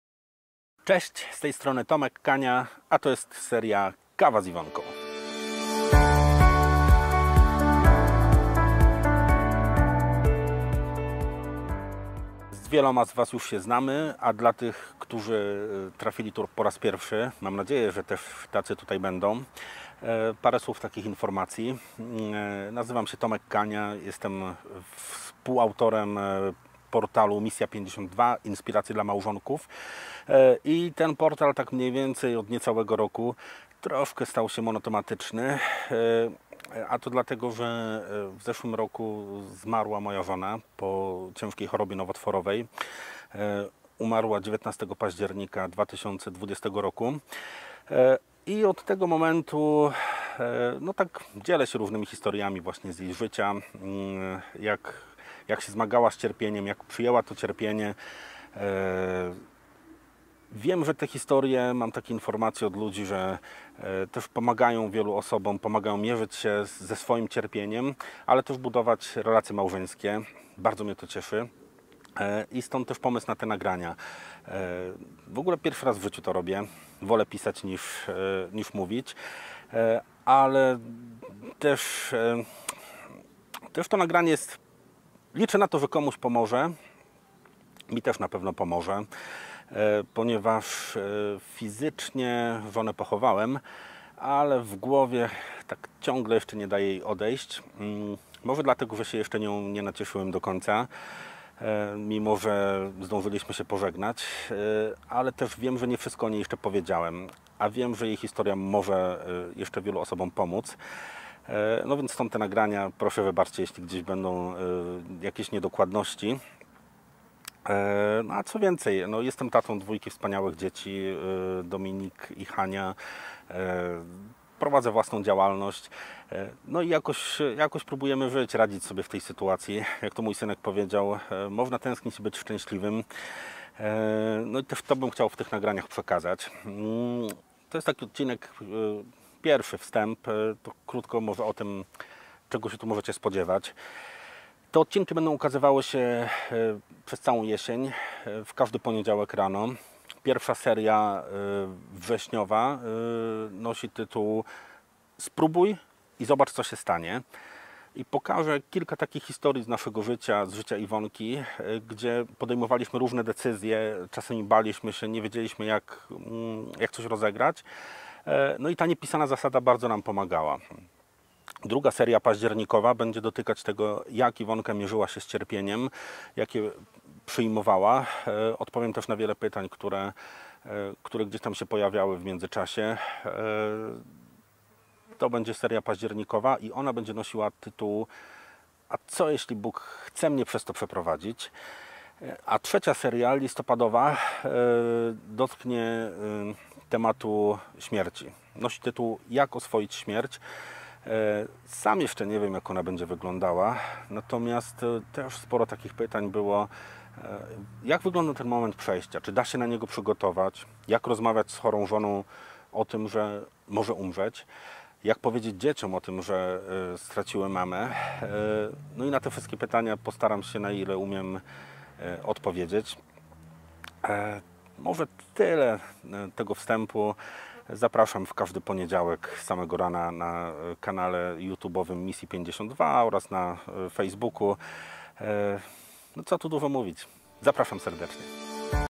KAWA Z IWONKĄ [#0] Zaczynamy?
Jesienna seria KAWA z IWONKĄ to kilkanaście krótkich historii o mojej żonie, Iwonce, która odeszła 19 października 2020 roku.  Krótkie odcinki ukazywać się będą w każdą niedzielę o 20:00 i podzielone zostały na 4 serie:

WRZEŚIEŃ / Spróbuj i zobacz co się stanie
PAŹDZIERNIK / Co, jeśli Bóg chce cię przez to przeprowadzić?
LISTOPAD / Jak oswoić śmierć
GRUDZIEŃ / Jak żyć po stracie

Zapraszam
Tomek Kania, mąż Iwonki